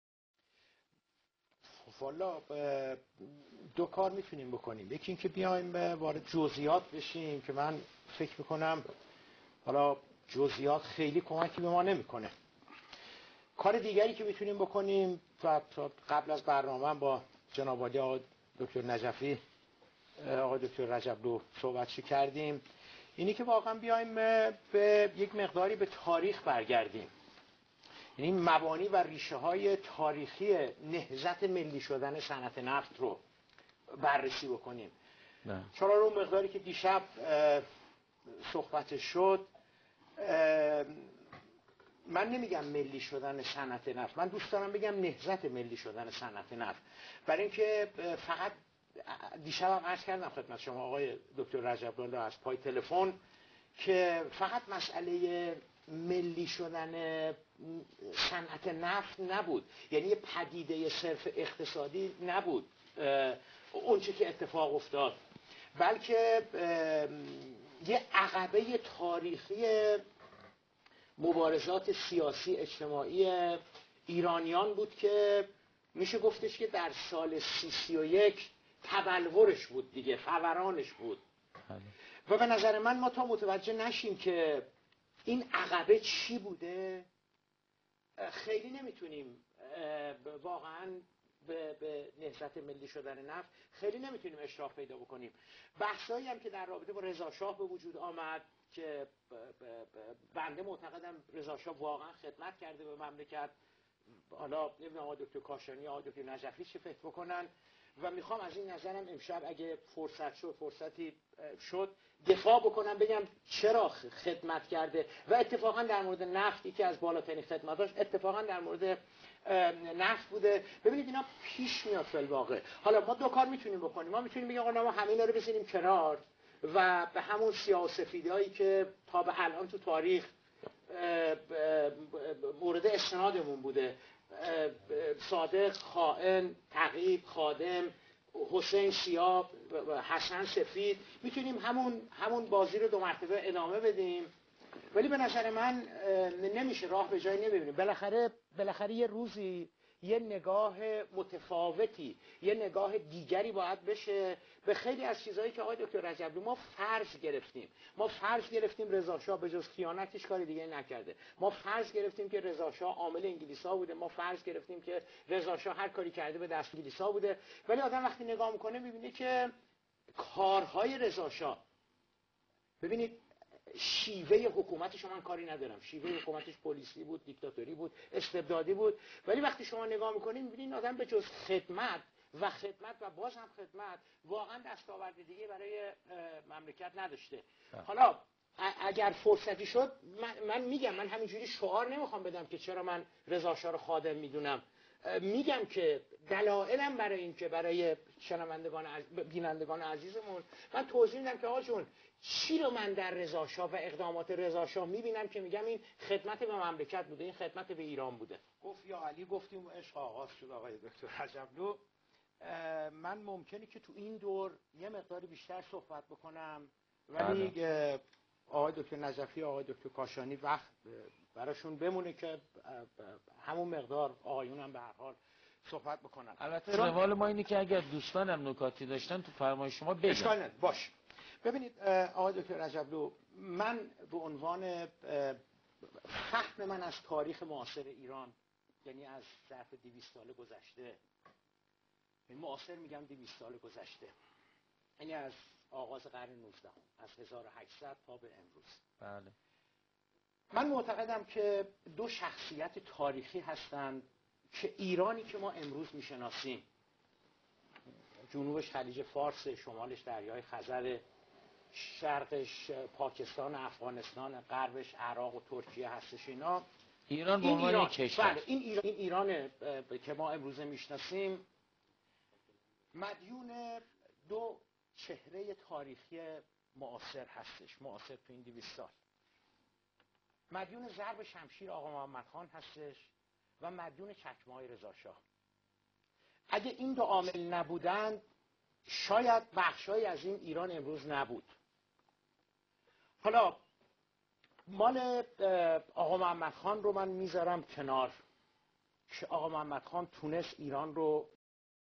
Iran Sadegh Ziba Kalam on Reza Shah Pahlavi (I) contributions
نظر صادق زیباکلام درباره «خدمات» رضاشاه پهلوی ...